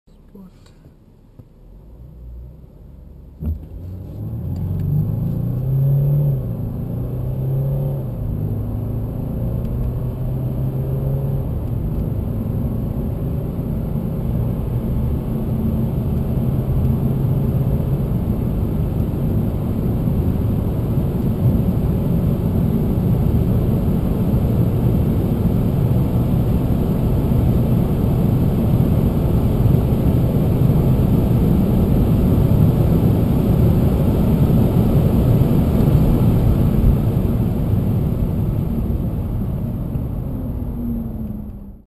Mercedes-Benz CLS 350 BlueTEC facelift acceleration 200 km/h (2014)
Sprint Mercedes-Benz CLS 350 BlueTEC facelift (9 speed automatic gearbox)
0-200 km/h

Official data:
3.0 l diesel engine, V6
258 HP, 620 Nm
0-100 km/h: 6.5 s